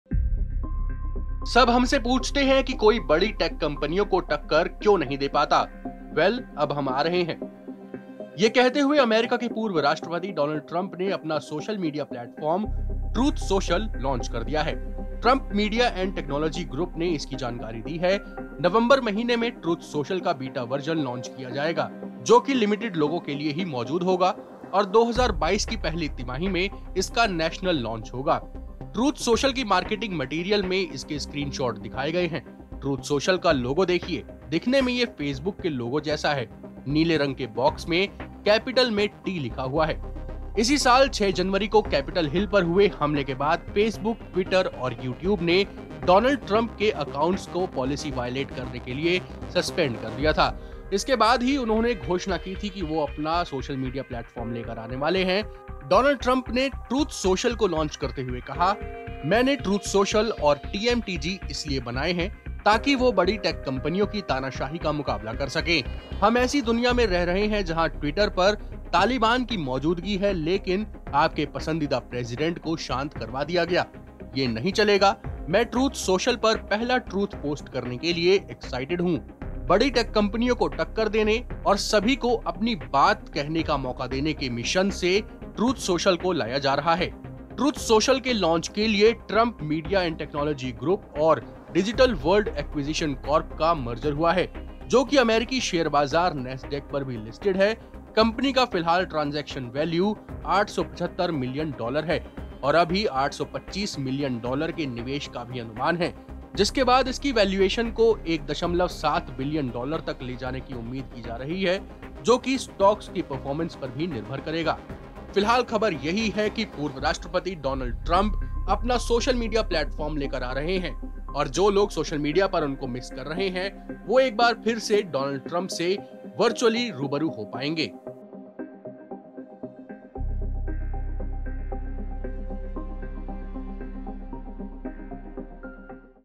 Donald Trump को Facebook, Youtube और Twitter ने किया हुआ है बैन, खुद बनाया Social Media प्लैटफॉर्म
अमेरिका के पूर्व राष्ट्रपति डॉनल्ड ट्रंप ने अपना सोशल मीडिया प्लैटफॉर्म 'ट्रूथ सोशल' लॉन्च कर दिया है. ट्रंप मीडिया एंड टेक्नोलॉजी ग्रुप ने इसकी जानकारी दी है. नवंबर महीने में ट्रूथ सोशल का बीटा वर्जन लॉन्च किया जाएगा जो कि लिमिटेड लोगों के लिए ही मौजूद होगा और 2022 की पहली तिमाही मे इसका नेशनल लॉन्च होगा.